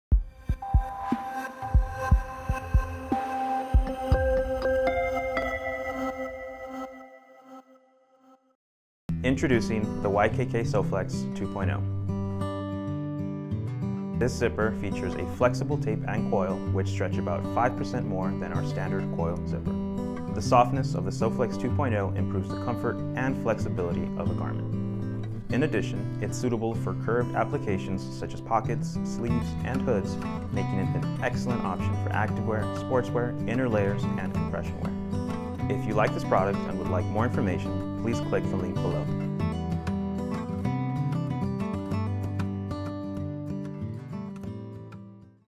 【YKK OFFICIAL】SOFLEX® 2.0 Introduction
【About YKK Digital Showroom】
YKK has launched a digital showroom as a means to better engage with customers, virtually, regardless of time or location. The concept behind the online showroom is “connection,” a play on YKK’s expertise as a manufacturer of fastening products. The space features six floors, each with a different theme. Here's a sneak peak of our "trend" floor products' introduction. Visit the showroom to see more!

【About Trend Product】
SOFLEX® 2.0 is a stretchy coil zipper that stretches along with the fabric. Not only is the tape stretchable, but the coil elements as well! SOFLEX® 2.0 features a stretch rate of about 5% by 1KG load, based on internal testing, and is a great option for compression garments or other lightweight garments using highly stretchable fabrics.